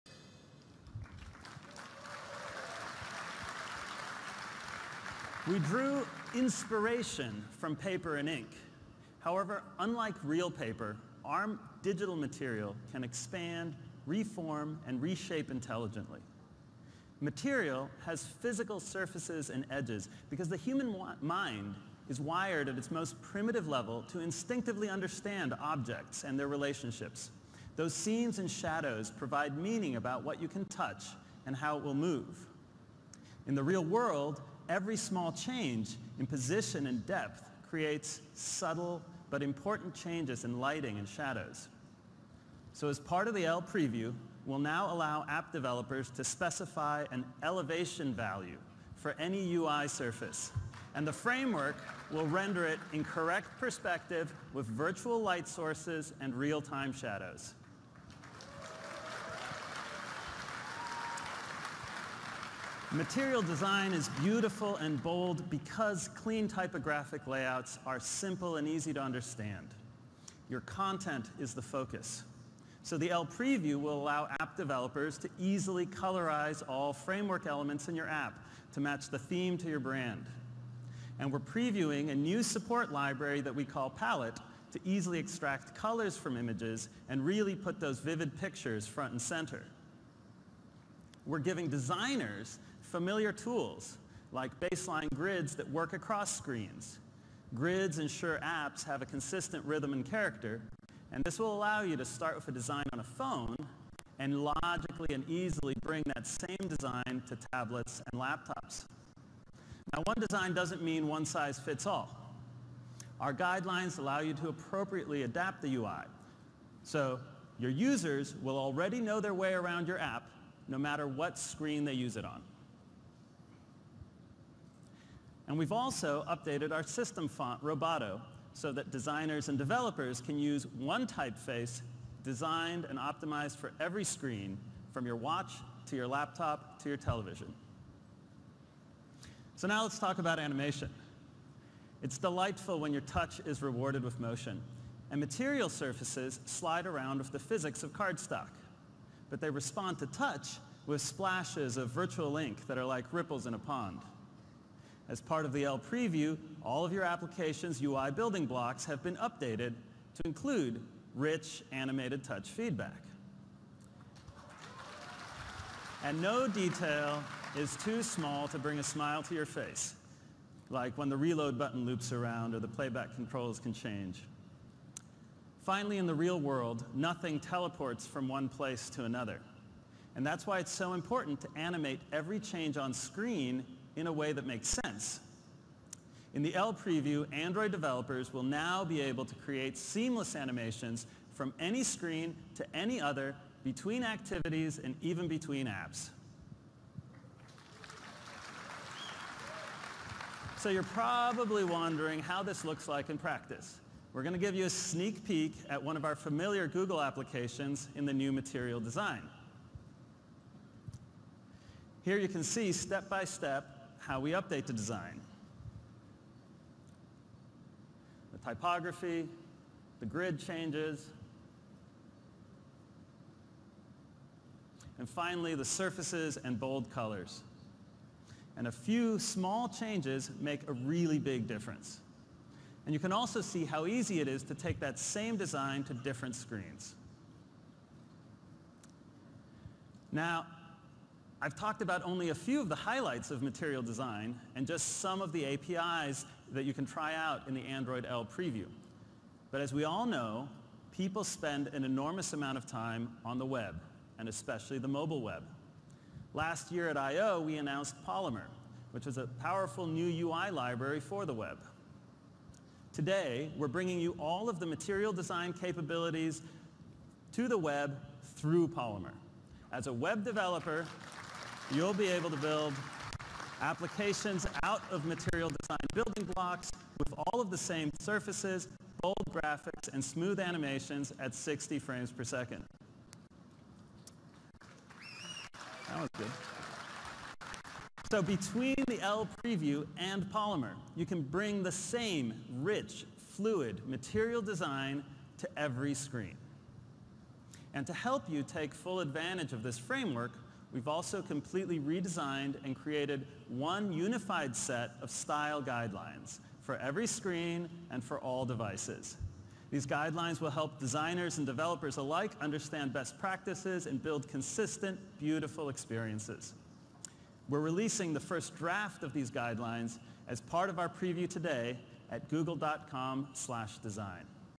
Google IO 2014 Keynote Live Stream - Material Design Part 2
Material Design - Part 2

3:36 - Gmail on Android L
4:26 - Material Design for the Web